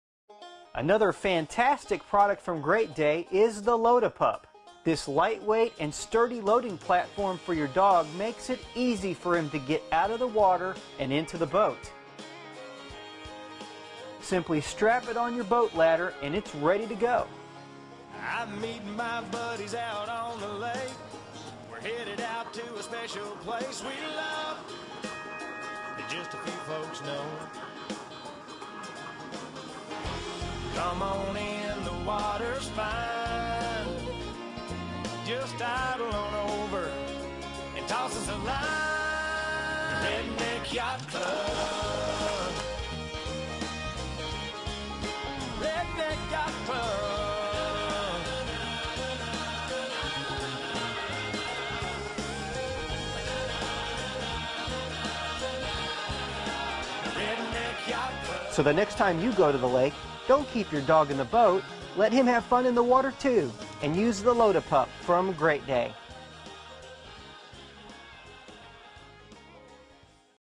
Load A Pup
Your dog will never again sit home while you enjoy a day on the water. With the Load-A-Pup, your dog will enjoy all the fun associated with recreational boating. Simply hook the Load-A-Pup to your boat's boarding ladder, and your dog can enter and leave the boat without your assistance. No more bending over or tugging at wet fur! Constructed of lightweight aluminum, the Load-A-Pup has gripping edges to ensure solid, safe footing. Powder-coated finish provides years of service. Comes with security straps. Folds flat for storage. 14" x 20" platform.